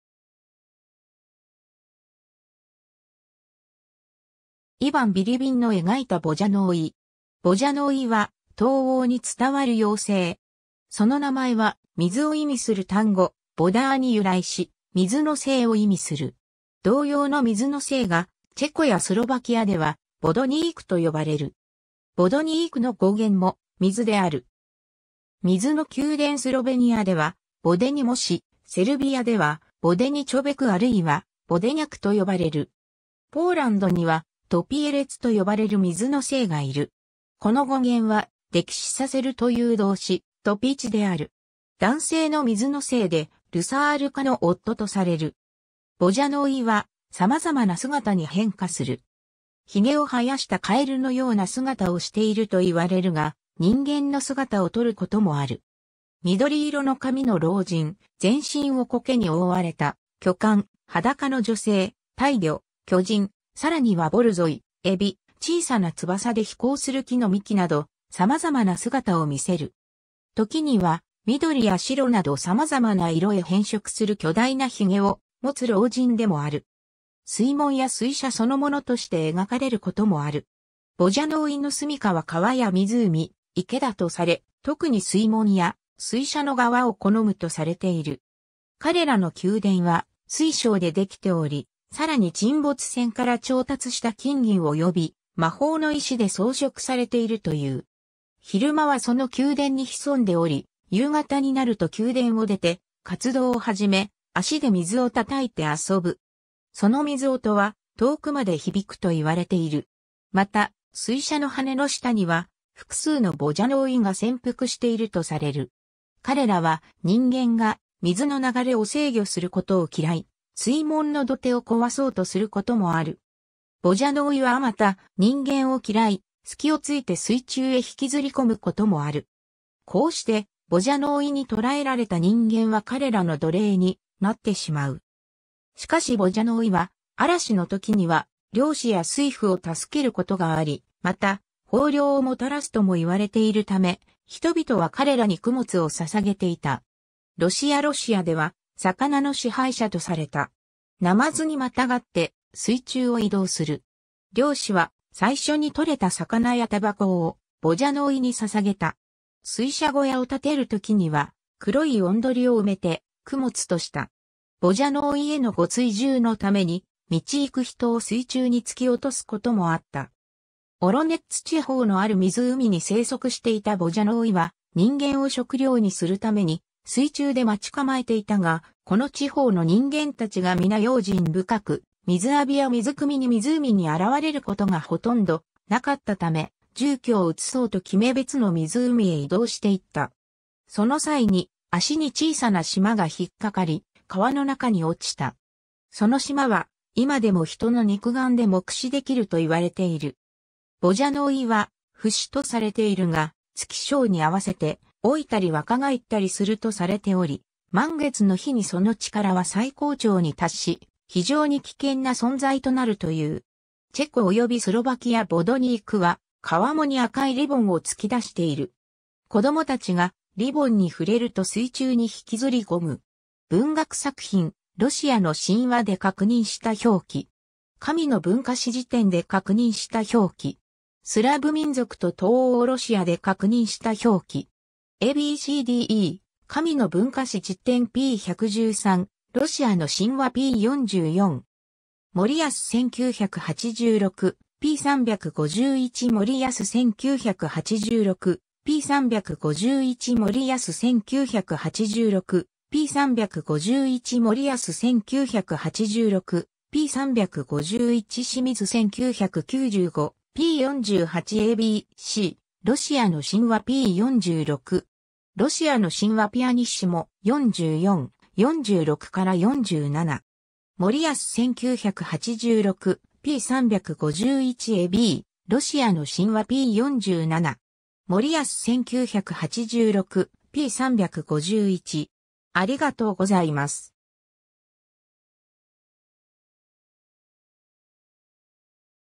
ヴォジャノーイ
神話・伝説の水生生物
イヴァン・ビリビンの描いたヴォジャノーイ。
 ヴォジャノーイ（ヴォヂャノーイ、ヴォジャノイとも。ロシア語: водяной ラテン文字表記例 Vodyanoy, Vodianoi, Vodjanoj、ウクライナ語: водяник）は、東欧に伝わる妖精。
その名前は、水を意味する単語「ヴォダー (voda)」に由来し、「水の精」を意味する。
 同様の水の精が、チェコやスロヴァキアでは、ヴォドニークと呼ばれる。
 ヴォドニークの語源も「水（ヴォダ）」である。
水の宮殿 スロヴェニアではヴォデニ・モシ、セルビアではヴォデニ・チョベクあるいはヴォデニャクと呼ばれる。
 ポーランドには、トピェレツと呼ばれる水の精がいる。
この語源は「溺死させる」という動詞「トピチ」である。
 男性の水の精で、 ルサールカの夫とされる。
 ヴォジャノーイは様々な姿に変化する。
 髭を生やしたカエルのような姿をしているといわれるが、人間の姿をとることもある。
緑色の髪の老人、全身を苔に覆われた巨漢、裸の女性、大魚、巨人、さらには ボルゾイ、海老、小さな翼で飛行する木の幹など、様々な姿を見せる。
時には、緑や白など様々な色へ変色する巨大な髭をもつ老人でもある。
 水門や水車そのものとして描かれることもある。
 ヴォジャノーイの住み処は川や湖、池だとされ、特に水門や水車の側を好むとされている。
彼らの宮殿は水晶で出来ており、さらに沈没船から調達した金銀および魔法の石で装飾されているという。
昼間はその宮殿に潜んでおり、夕方になると宮殿を出て活動を始め、脚で水を叩いて遊ぶ。
その水音は遠くまで響くといわれている。
また、水車の羽根の下には、複数のヴォジャノーイが潜伏しているとされる。
彼らは人間が水の流れを制御することを嫌い、水門の土手を壊そうとすることもある。
 ヴォジャノーイはまた、人間を嫌い、隙をついて水中へ引きずり込むこともある。
こうしてヴォジャノーイに捕らえられた人間は彼らの奴隷になってしまう。
しかしヴォジャノーイは、嵐の時には漁師や水夫を助けることがあり、また、豊漁をもたらすとも言われているため、人々は彼らに供物を捧げていた。
 ロシア ロシアでは、魚の支配者とされた。
ナマズにまたがって水中を移動する。
漁師は、最初にとれた魚や煙草をヴォジャノーイに捧げた。
水車小屋をたてるときには、黒い雄鶏を埋めて供物とした。
ヴォジャノーイへの御追従のために、道行く人を水中に突き落とすこともあった。
 オロネッツ地方のある湖に生息していたヴォジャノーイは、人間を食料にするために水中で待ち構えていたが、この地方の人間達が皆用心深く、水浴びや水汲みに湖に現れることが殆どなかったため、住居を移そうと決め別の湖へ移動していった。
その際に、脚に小さな島が引っかかり、河の中に落ちた。
その島は今でも人の肉眼で目視できると言われている。
 ヴォジャノーイは不死とされているが、月相に合わせて老いたり若返ったりするとされており、 満月の日にその力は最高潮に達し、非常に危険な存在となるという。
 チェコおよびスロヴァキア ヴォドニークは川面に赤いリボンを突き出している。
子供たちがリボンに触れると水中に引きずり込む。
 文学作品 ^ 『ロシアの神話』で確認した表記。
 ^ 『神の文化史事典』で確認した表記。
 ^ 『スラヴ民族と東欧ロシア』で確認した表記。
 ^ a b c d e 『神の文化史事典』p. 113（ヴォヂャノーイの項） ^ 『ロシアの神話』p. 44（「スラヴの神話」）。
 ^ 森安 1986, p. 351 ^ 森安 1986, p. 351 ^ 森安 1986, p. 351 ^ 森安 1986, p. 351 ^ 清水 1995, p. 48 ^ a b c 『ロシアの神話』p. 46（「スラヴの神話」）。
 ^ 『ロシアの神話』pp. 44, 46-47（「スラヴの神話」）。
 ^ 森安 1986, p. 351 ^ a b 『ロシアの神話』p. 47（「スラヴの神話」）。
 ^ 森安 1986, p. 351